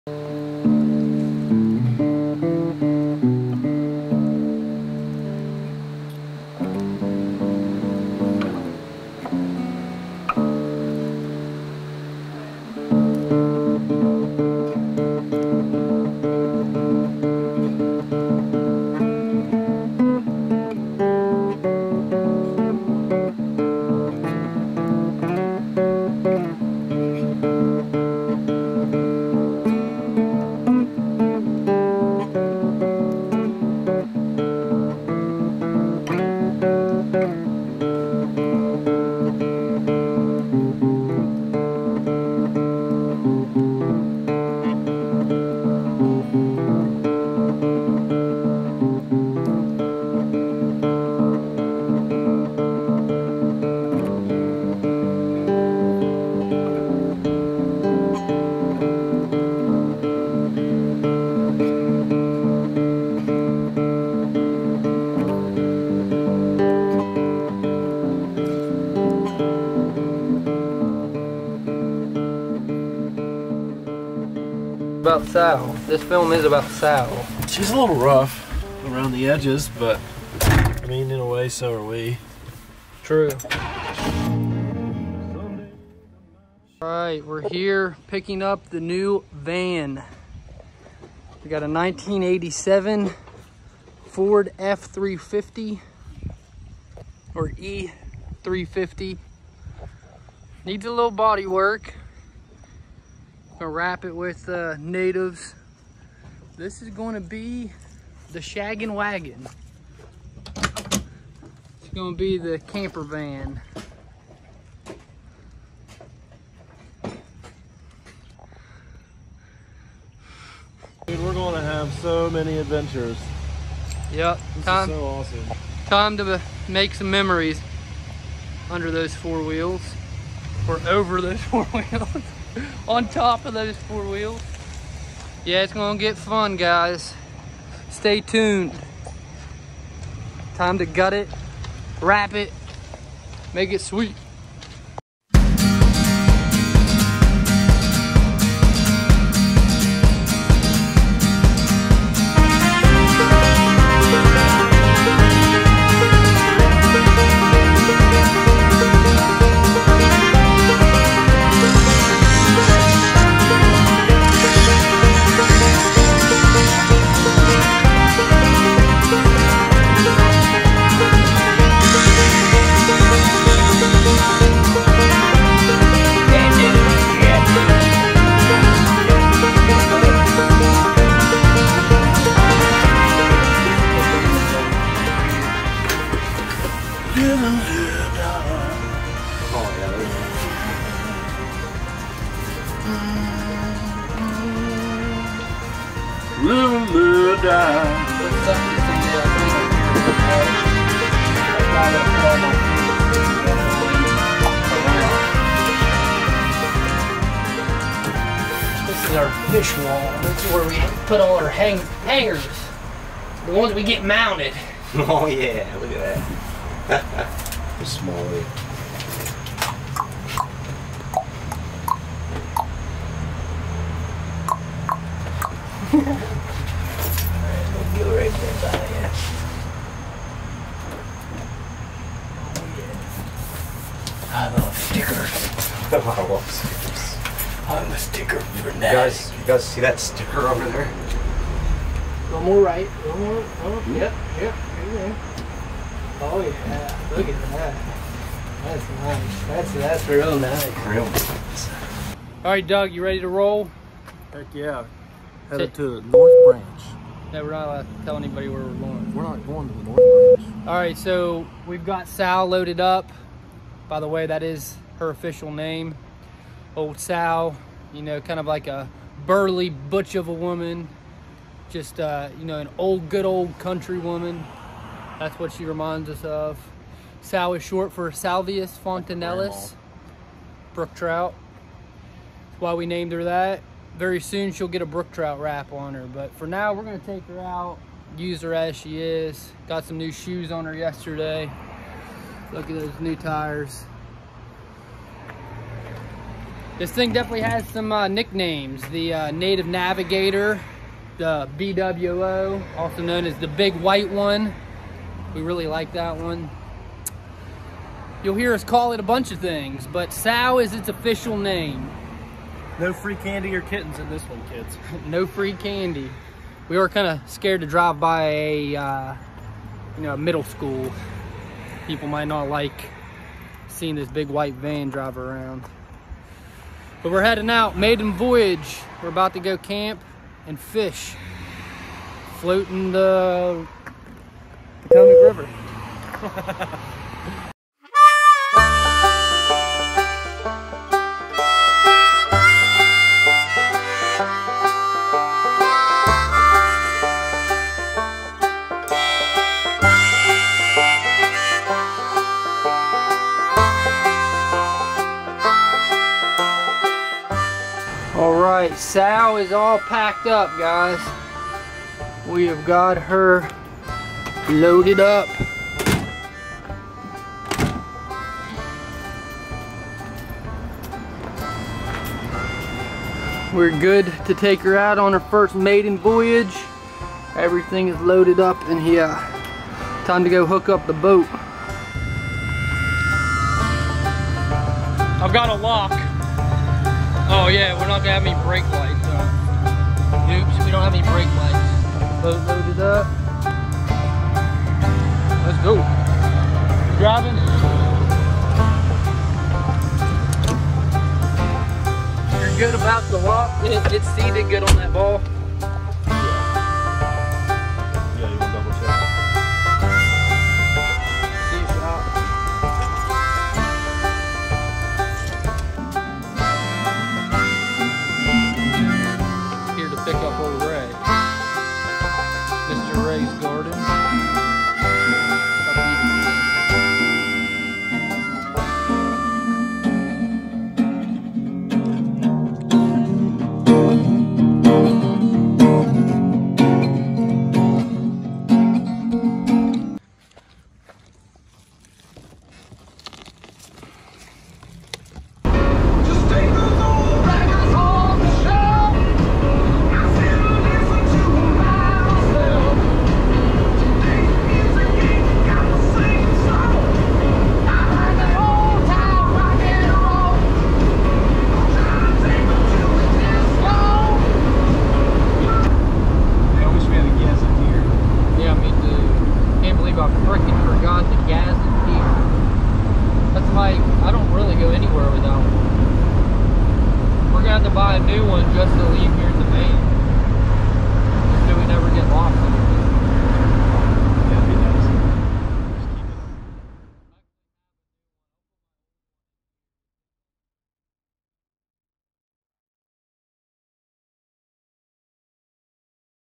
Adventures In Sal VOL. 1 - The Natives Fish Wagon Fly Fishing Ambulance Van Conversion
We bought a 1987 Ford E350 to get us to and from our fly fishing destinations. Well we hope that's the case. The beginning is bright behind this old girl we named SAL after Salvelinus fontinalis - Brook Trout. Fly Fishing Stealth Camping at its finest!